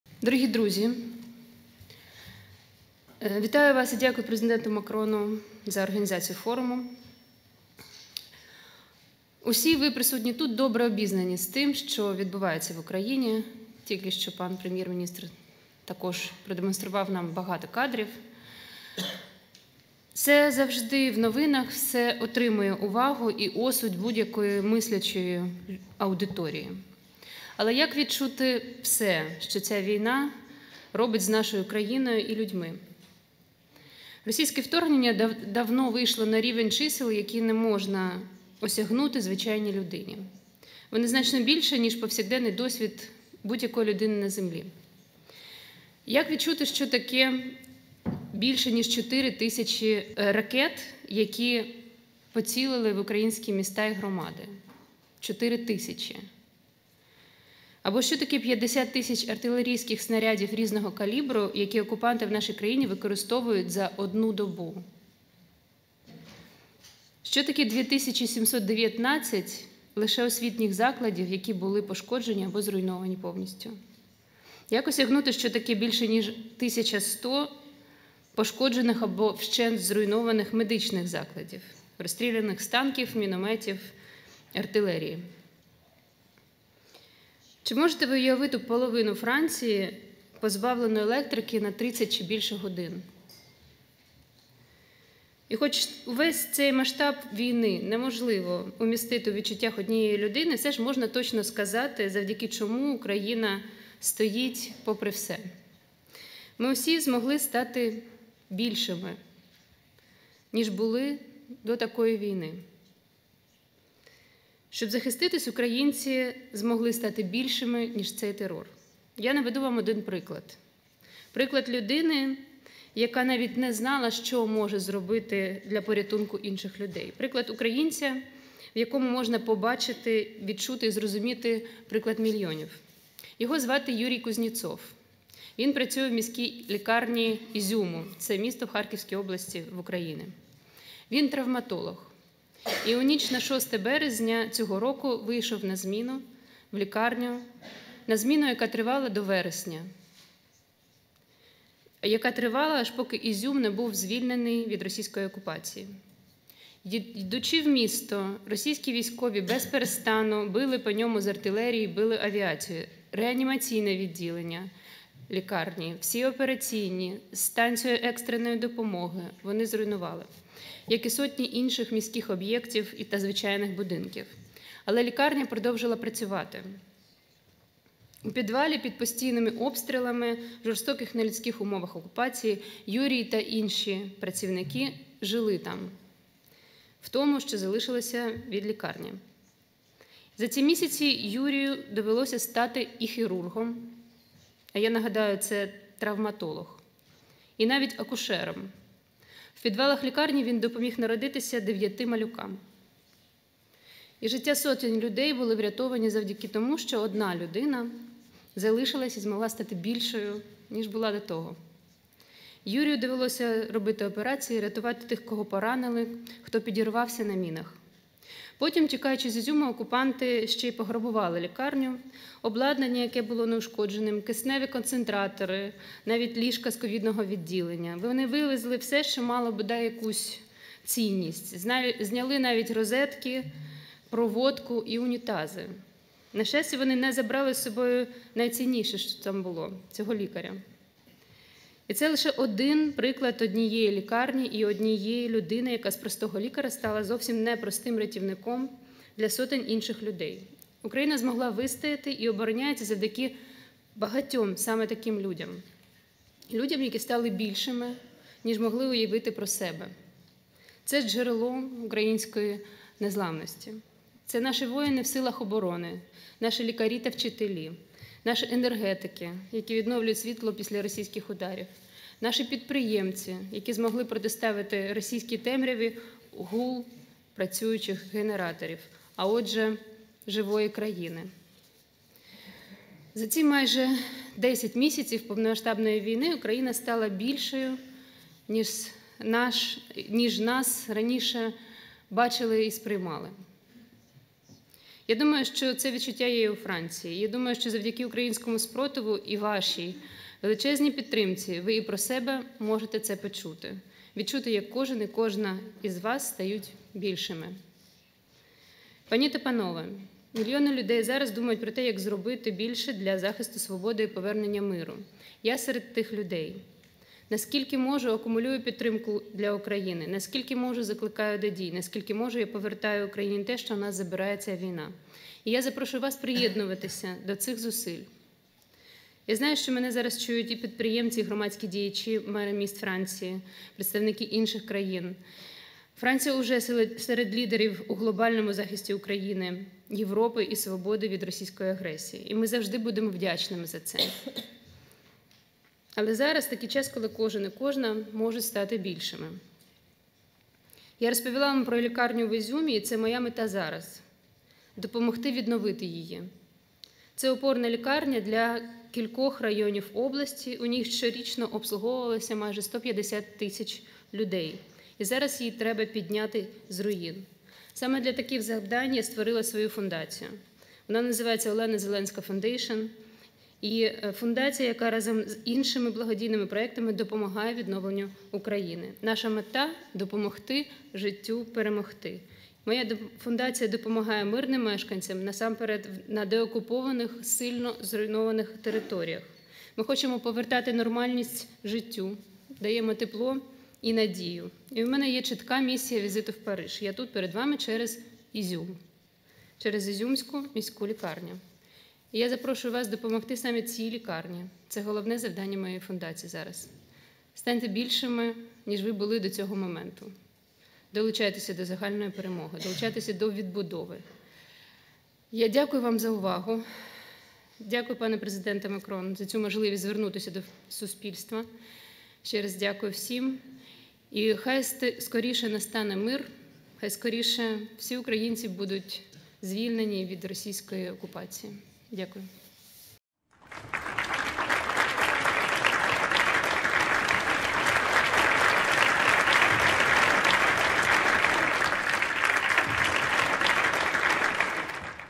Візит Першої Леді у Францію
Його звати Юрій Кузнєцов. Він працював травматологом у міській лікарні Ізюма й у ніч на 6 березня просто вийшов на свою зміну. На зміну, яка тривала... до вересня, до самого звільнення з-під російської окупації. Разом із небагатьма іншими працівниками, які змогли залишитися, з містянами, які стали волонтерами, у зруйнованій лікарні, без тепла, електрики, води (щоб її отримати, розтоплювали сніг).

За ці місяці Юрію довелося стати і хірургом, і акушером: у підвалах лікарні народилися девʼять малюків. Сотні життів урятовані завдяки тому, що ця людина та її колеги залишились і зробили більше, ніж могли.

Увесь масштаб війни неможливо вмістити у відчуттях однієї людини, але можна точно сказати, завдяки чому Україна попри все стоїть: ми всі змогли стати більшими, ніж були до війни. Щоб захиститись, українці щодня роблять більше, ніж могли уявити самі.

Під час візиту до Франції розповіла про нашу країну – знеструмлену російськими ударами, понівечену обстрілами й окупацією, та все одно незламну – на Міжнародній конференції на підтримку громадянської стійкості в Україні у зимовий період. Захід ініційований Президентом Макроном.

Закликала присутніх допомогти ізюмській лікарні, її героїчним лікарям, котрі вже стільки пережили, відновитися, щоб рятувати далі.

Допомогти їм – це також можливість стати більшими. Бо допомагати й рятувати – завжди означає рости над собою.